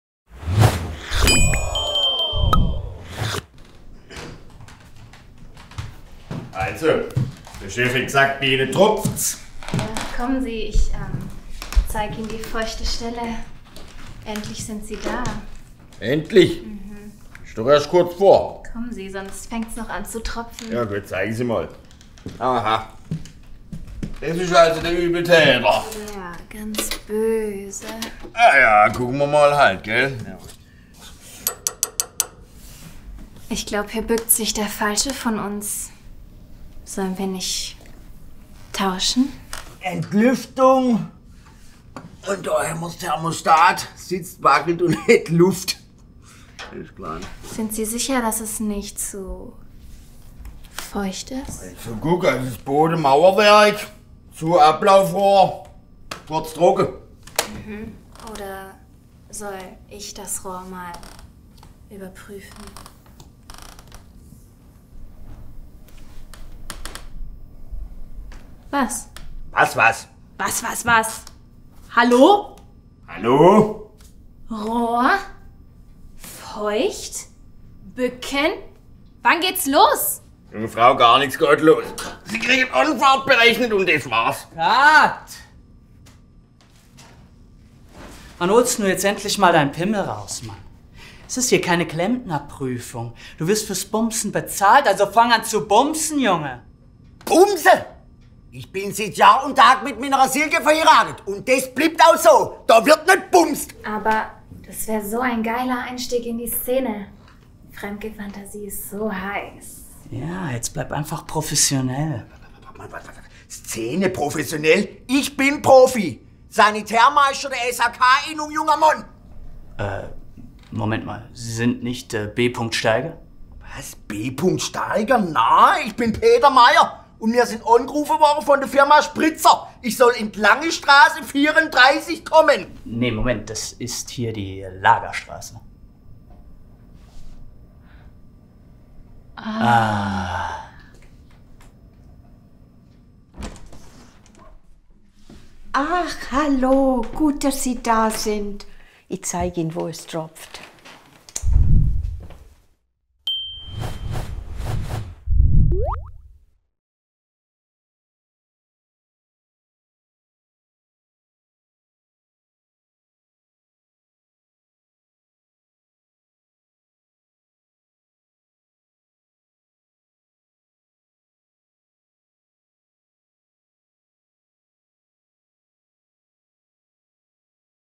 Wenn der Handwerker kommt... 👨🏾‍🔧🍑 | Kennt jeder! | mit Cossu & Alina Khani
Handwerker sind überall gesucht. Und jeder hat so sein Spezialgebiet. Deshalb am besten immer den richtigen Handwerker für das jeweilige Problem engagieren. So wird auch das RICHTIGE „repariert“.